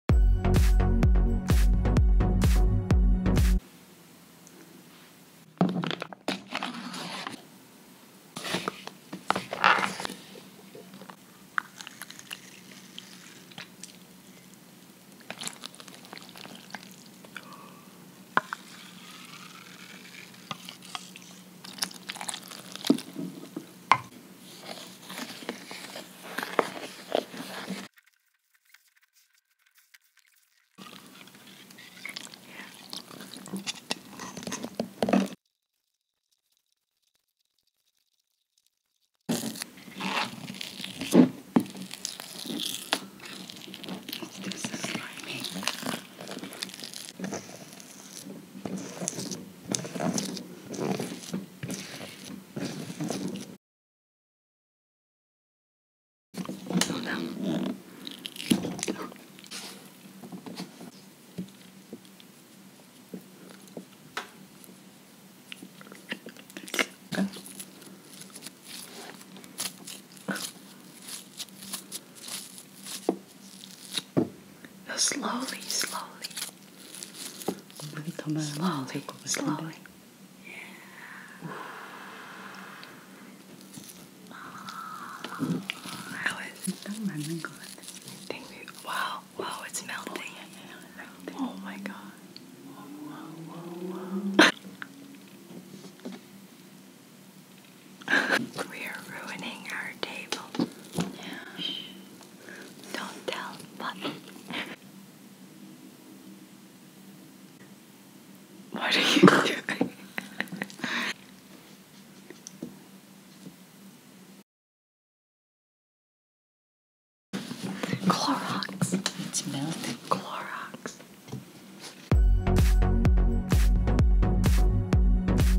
EDIBLE UNICORN SLIME (ASMR) | Kim&Liz Too
Hi guys! Today, we are trying out PINK EDIBLE UNICORN SLIME!
Hope you guys enjoyed (if you did, please give this video a thumbs up)